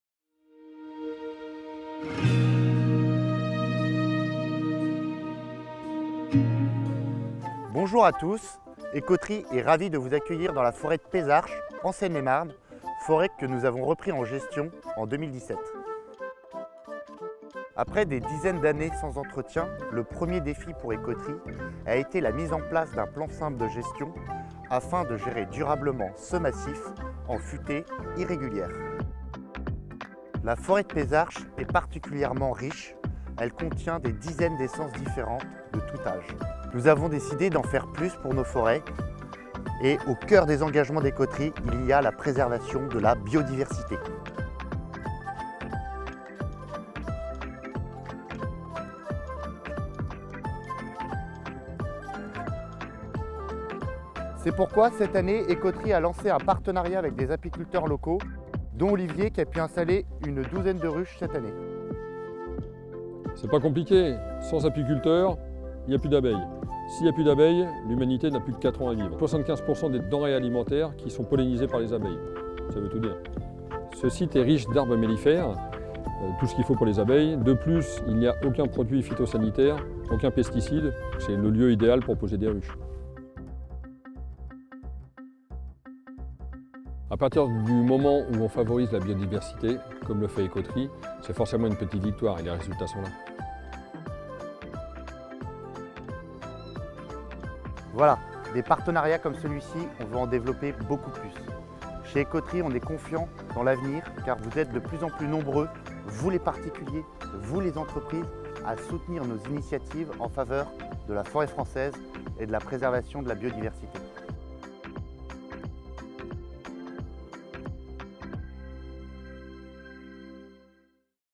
Découvrez la forêt de Pézarches en Seine-et-Marne avec Baudouin | EcoTree
Envie d'en savoir plus ? Plus d'infos sur la forêt de Pézarches et l'ensemble de nos parcelles